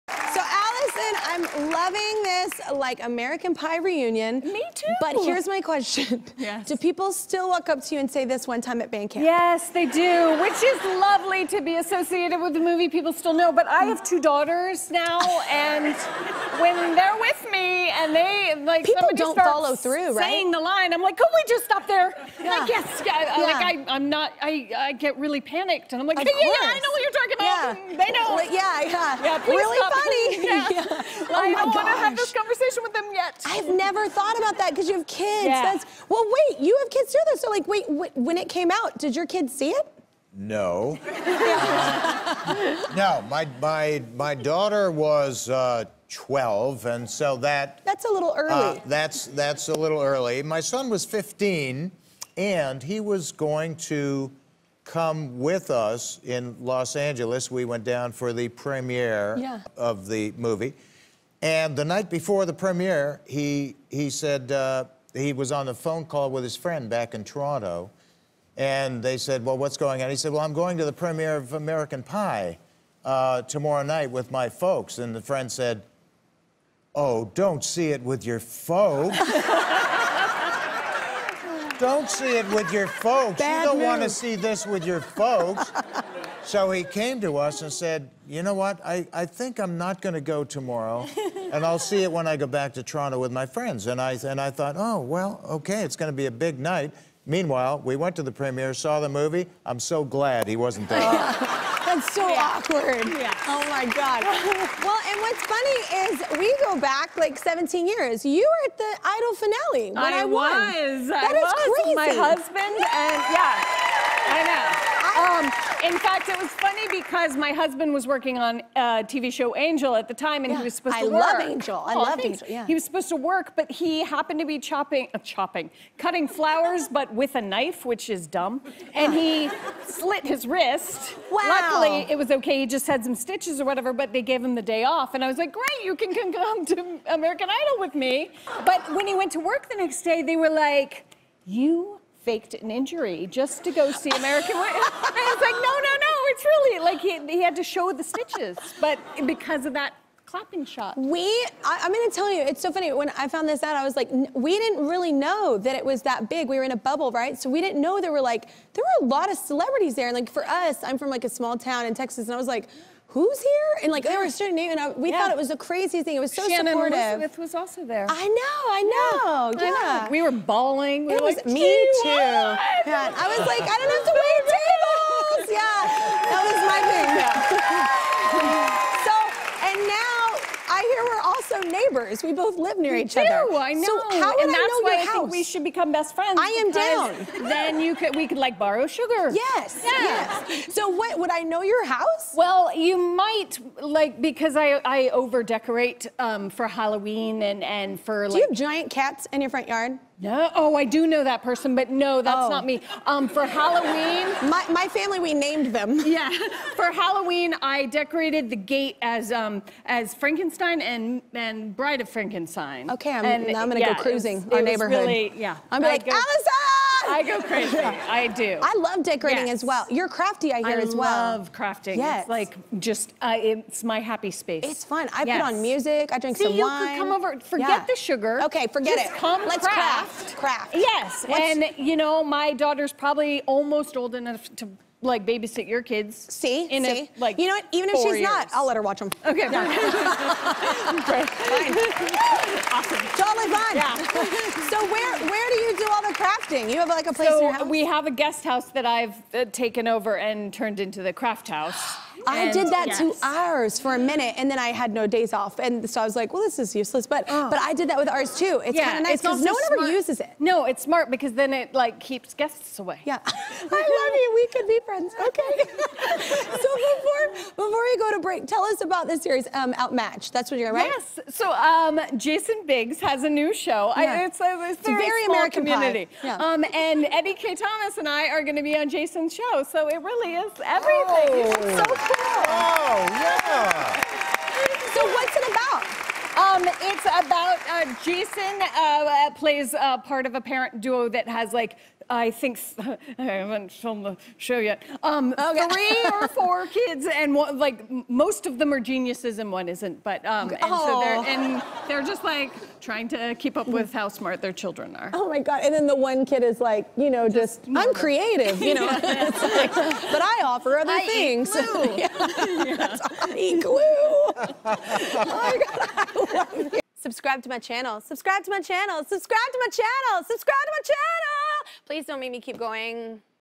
Alyson Hannigan's Husband Accused Of Faking Injury While Spotted In 'American Idol' Audience
Alyson Hannigan was totally in the audience when Kelly Clarkson won “American Idol.” Alyson remembers being in the audience during Kelly’s career-making moment and also shares a funny story about how her husband almost got in serious trouble with his job after getting caught on camera on the show.